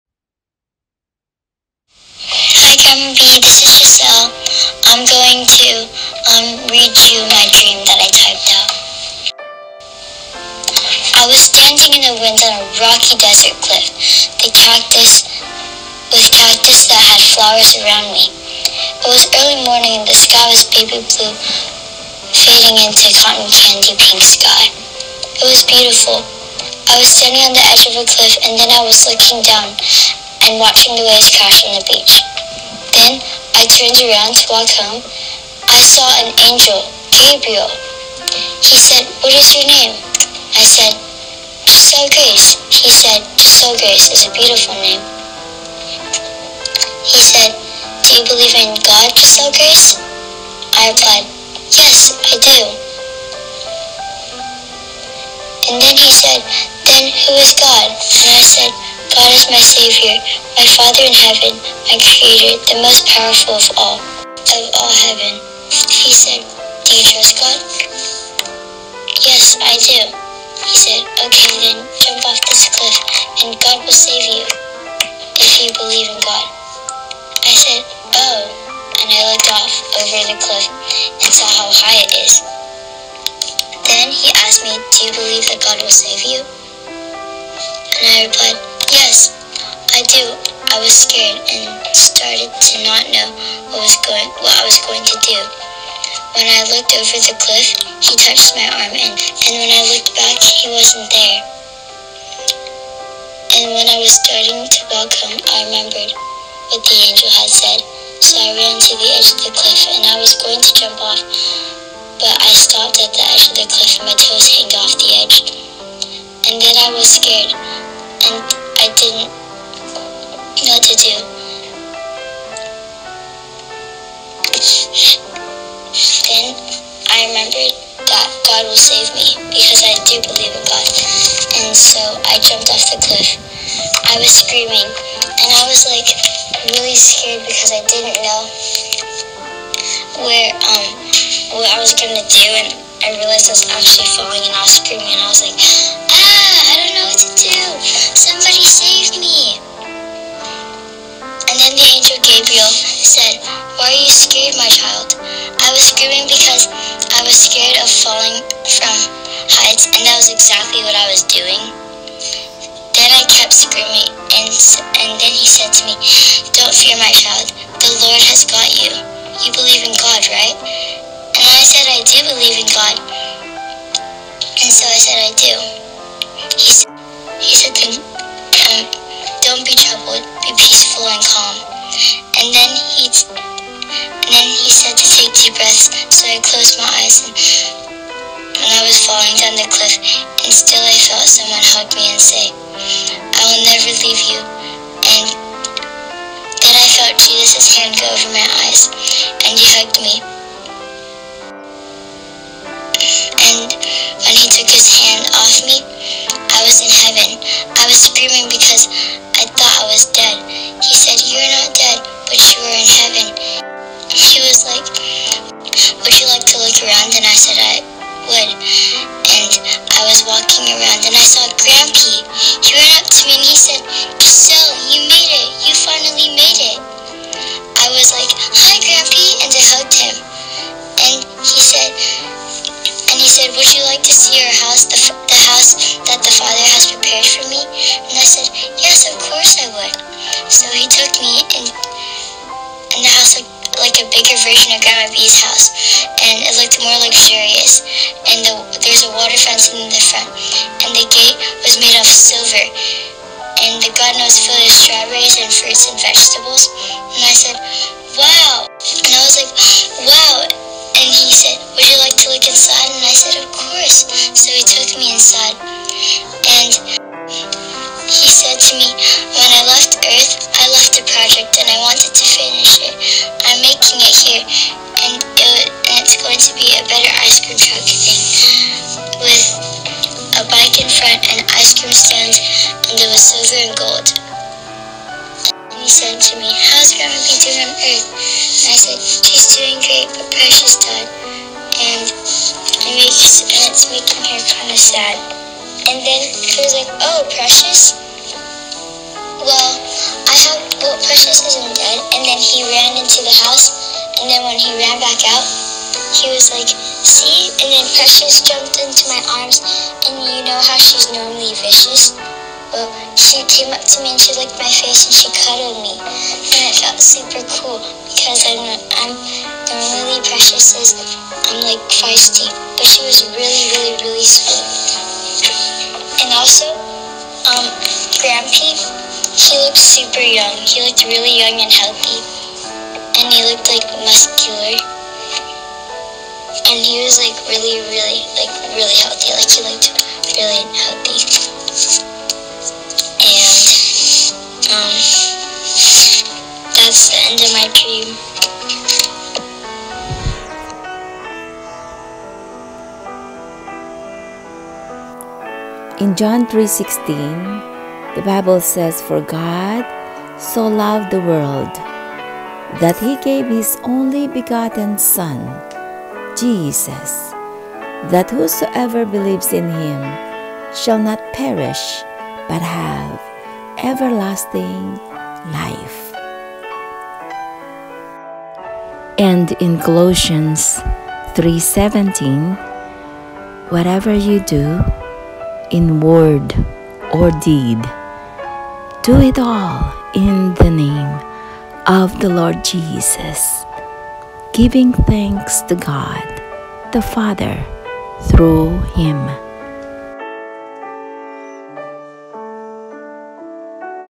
Girl Has Dream She Met An Angel & Jesus, Went To Heaven And Saw Grandpa And His Pet Dog 🐶
Bible Verse about Jacelle’s dream :

Angel Gabriel : Luke 1:19 - “And the angel answering said unto him, I am Gabriel, that stand in the presence of God; and am sent to speak unto thee, and to shew thee these glad tidings.”

What is your name ? Isaiah 43:1
“But now thus says the Lord, he who created you, O Jacob, he who formed you, O Israel: “Fear not, for I have redeemed you; I have called you by name, you are mine.”

Believe in God : John 14:1 "Do not let your hearts be troubled. You believe in God; believe in Me (Jesus) as well."

Who is God ? Psalm 68:19-20: "Praise be to the Lord, to God our Savior, who daily bears our burdens. Our God is a God who saves; from the Sovereign LORD comes escape from death."

Do you trust God? Psalm 28:7
“The Lord is my strength and my shield; in him my heart trusts, and I am helped; my heart exults, and with my song I give thanks to him.”

Do you believe that God will save you?

Acts 2:21 “And it shall come to pass that everyone who calls upon the name of the Lord shall be saved.”

Do not fear : Isaiah 41:10 “So do not fear, for I am with you; do not be dismayed, for I am your God. I will strengthen you and help you; I will uphold you with my righteous right hand.”

Do not be troubled :
John 14:1
“Do not let your hearts be troubled. You believe in God[a]; believe also in me. “

I will never leave you :
Hebrew 13:5 “Never will I leave you; never will I forsake you.”

Who Is Jesus ? Matthew 16:16 Simon Peter answered, "You are the Christ, the Son of the living God."

Heaven : Revelation 22:1-5
“Then the angel showed me the river of the water of life, bright as crystal, flowing from the throne of God and of the Lamb through the middle of the street of the city; also, on either side of the river, the tree of life with its twelve kinds of fruit, yielding its fruit each month. The leaves of the tree were for the healing of the nations. No longer will there be anything accursed, but the throne of God and of the Lamb will be in it, and his servants will worship him. They will see his face, and his name will be on their foreheads. And night will be no more. They will need no light of lamp or sun, for the Lord God will be their light, and they will reign forever and ever.”

House in Heaven : 2 Corinthians 5:1
“For we know that if the earthly tent we live in is destroyed, we have a building from God, an eternal house in heaven, not built by human hands.”

Pets in Heaven :

Job 12:10: "In his hand is the life of every living thing and the breath of all."